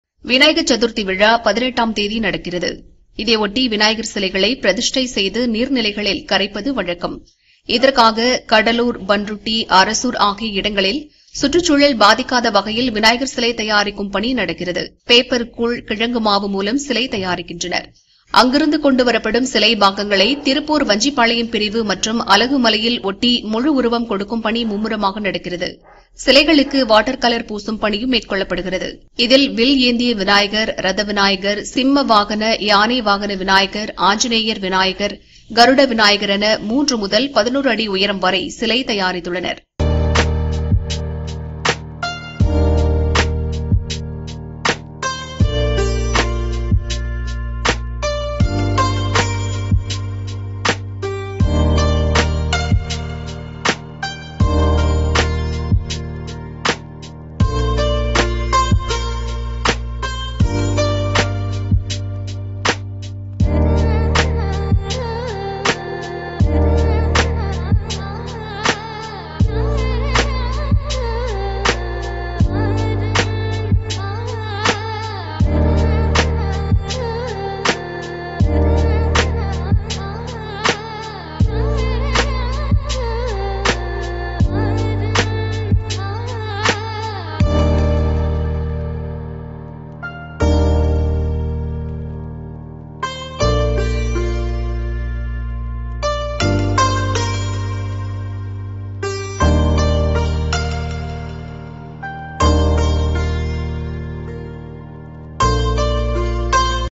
11 அடியில் பிரமாண்டமாக தயாராகும் விநாயகர் சிலைகள்
விநாயகர் சதுர்த்தி விழா 18ம் தேதி நடக்கிறது. இதையொட்டி விநாயகர் சிலைகளை பிரதிஷ்டை செய்து நீர்நிலைகளில் கரைப்பது வழக்கம்.

இதற்காக கடலுார், பண்ருட்டி, அரசூர் ஆகிய இடங்களில் சுற்று சூழல் பாதிக்காத வகையில் விநாயகர் சிலை தயாரிக்கும் பணி நடக்கிறது. பேப்பர் கூழ், கிழங்கு மாவு மூலம் சிலை தயாரிக்கின்றனர்.

அங்கிருந்து கொண்டு வரப்படும் சிலை பாகங்களை திருப்பூர் வஞ்சிபாளையம் பிரிவு மற்றும் அலகுமலையில் ஒட்டி முழு உருவம் கொடுக்கும் பணி மும்முரமாக நடக்கிறது. சிலைகளுக்கு வாட்டர் கலர் பூசும் பணியும் மேற்கொள்ளப்படுகிறது.

இதில் வில் ஏந்திய விநாயகர், ரத விநாயகர், சிம்ம வாகன, யானை வாகன விநாயகர், ஆஞ்சநேயர் விநாயகர், கருட விநாயகர் என 3 முதல் 11 அடி உயரம் வரை சிலை தயாரித்துள்ளனர்.